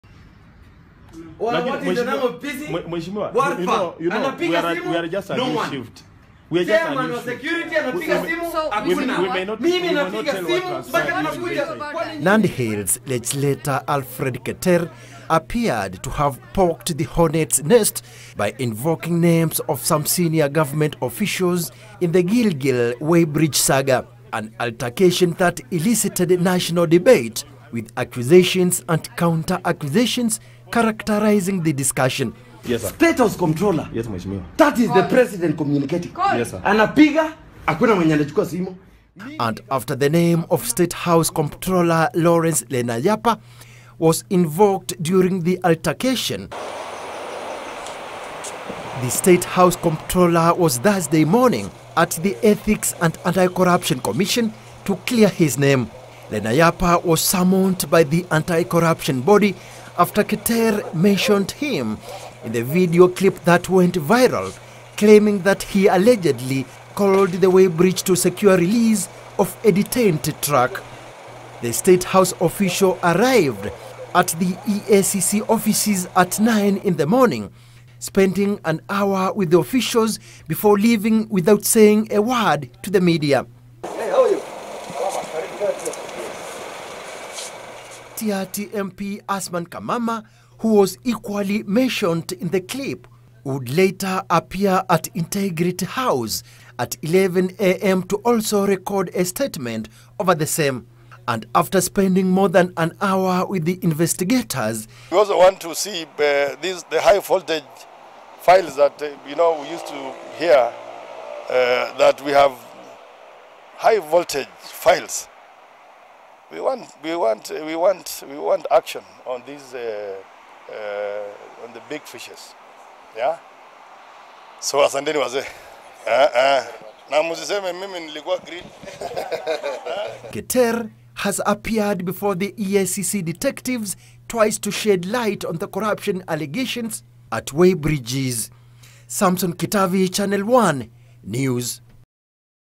EACC probes State House Comptroller over Keters saga
State House Comptroller Lawrence Lenayapa and Tiaty MP Asman Kamama honored summonses by the Ethics and Anti-Corruption Commission to record statements over recent altercation between Nandi Hills MP Alfred Keter and officers manning the Gilgil weighbridge. Lenayapa met EACC officials for an hour while Kamama who is also the chair of the House committee on security, appeared before the detectives an hour later. By Samson Kitavi.